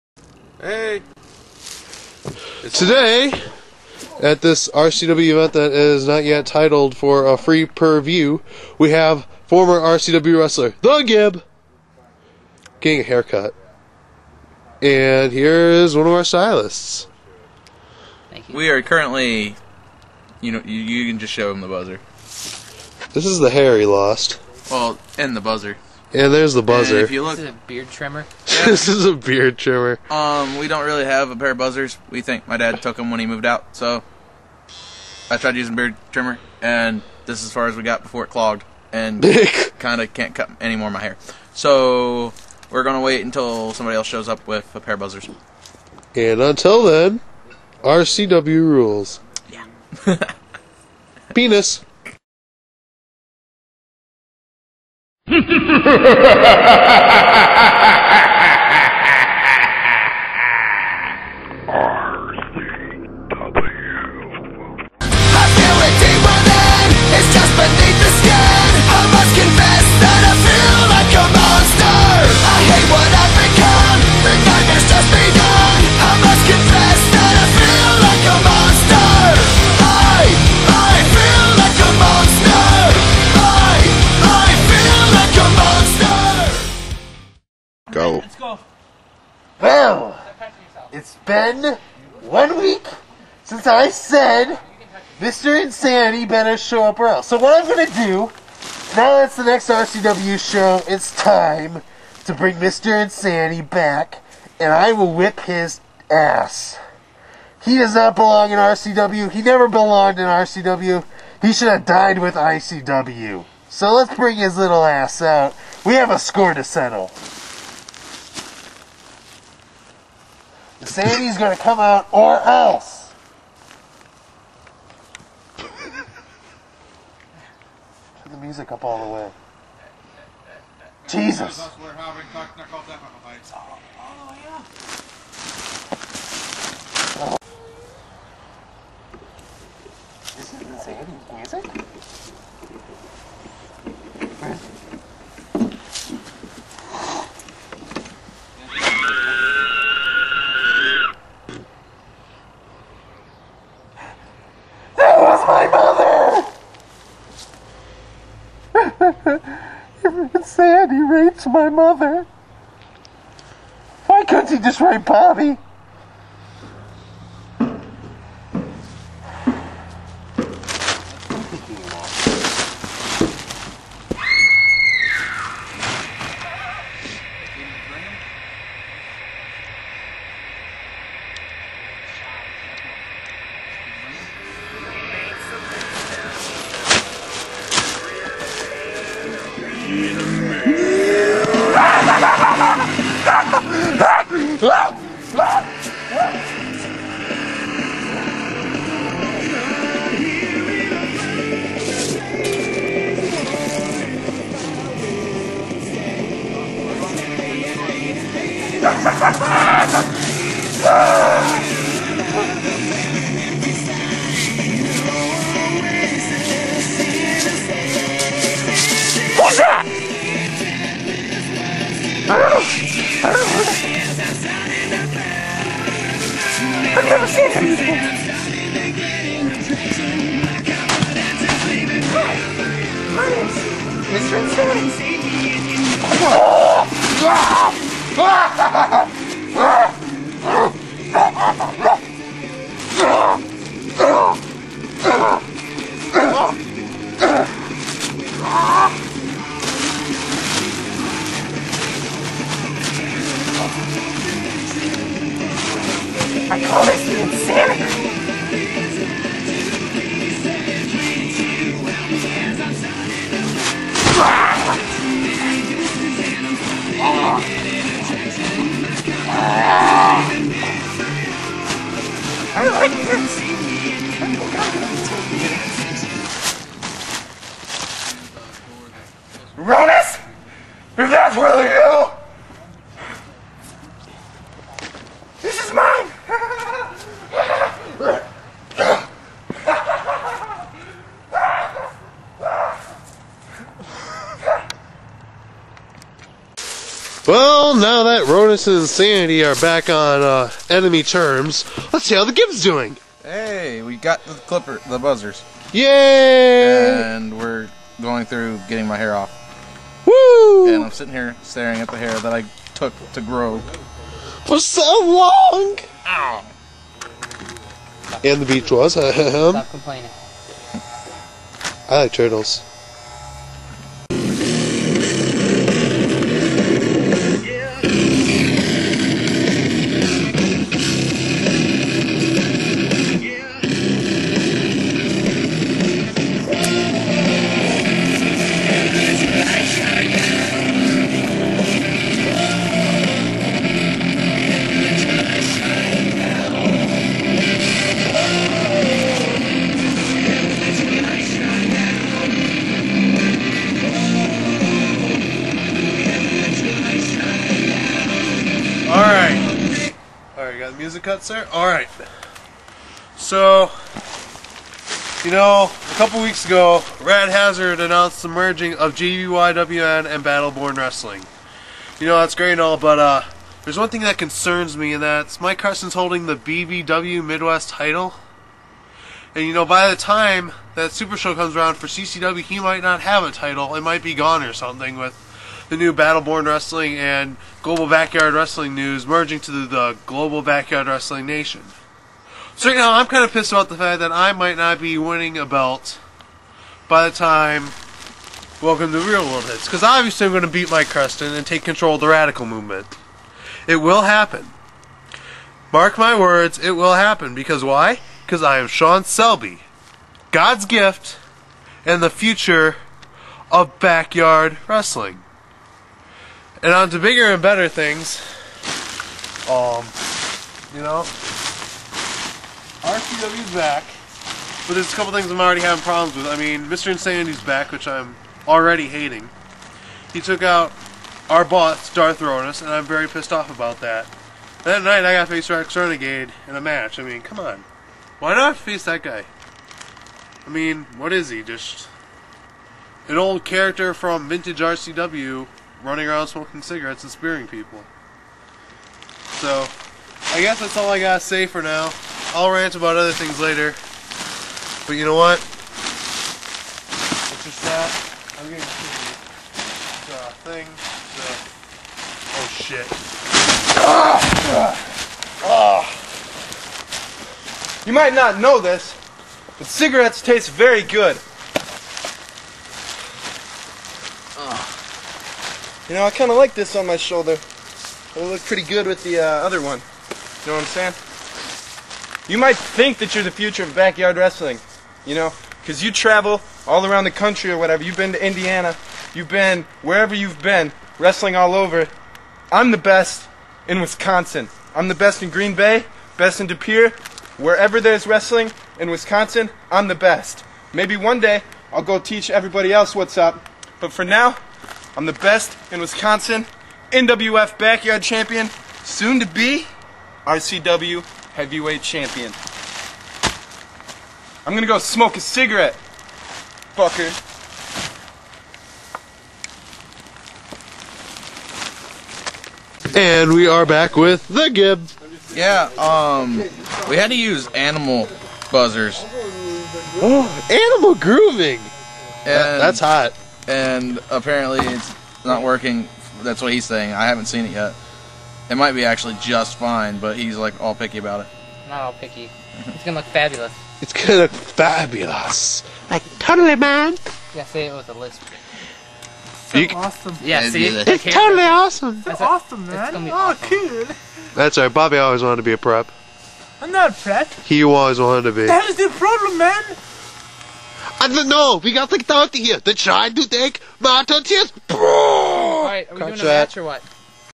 RCW Rulz Episode 3 (Part 1/3)
This part includes:
-The Gibb gets a haircut part 1
-RCW Intro
- Ronus/Insanity Confrontation
-The Gibb Gets a Haircut Part 2
-Shawn Selby Promo
-The Gibb Gets a Haircut Part 3